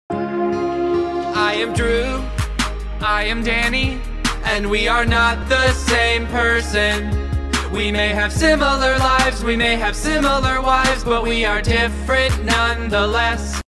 I am drew, I am danny! (meet Tinyability! read desc to know more about her!!)
so Tinyability is another one of my Roblox OCS, and she's based off of @Laughability!!